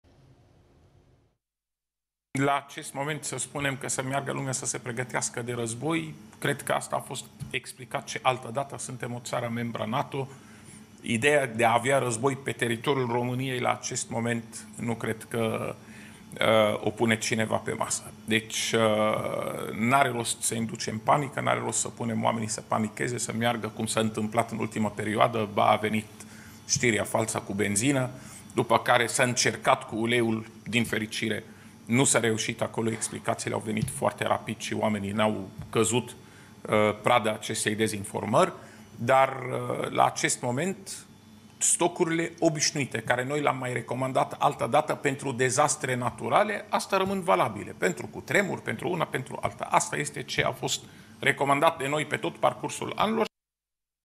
Raed Arafat: Ar trebui să avem în casă provizii pentru catastrofe naturale
Şeful DSU, Raed Arafat, a recomandat românilor, luni, să aibă în case stocurile obişnuite în caz de cutremur sau alte dezastre naturale, spunând că nu are sens să "inducem panică".